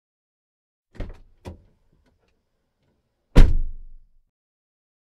Car Door Opening/Closing Sound Effect
Opening car door and letting it fall shut.